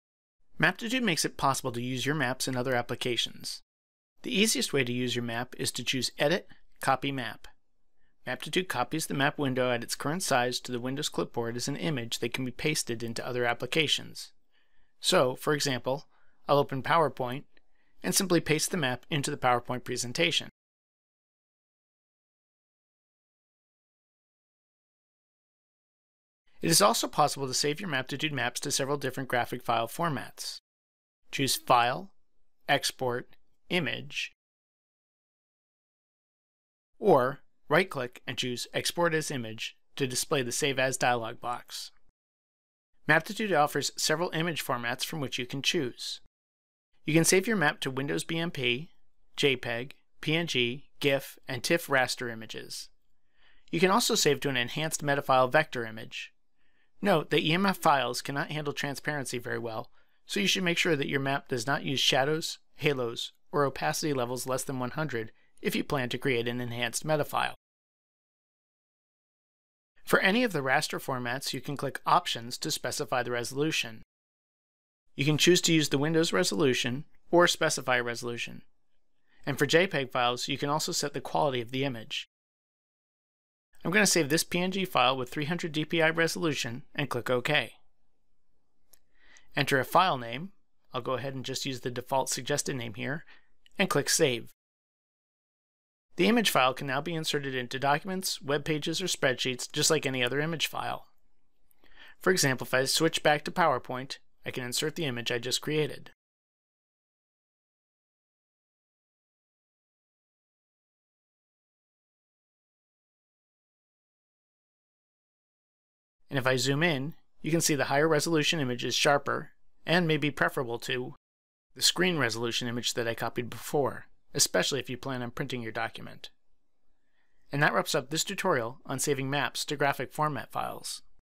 Maptitude 2019 Map Export & Map Saving to Images & PowerPoint Jpeg, PNG, BMP
Saving Maptitude maps to different graphic file formats and using Maptitude maps in other applications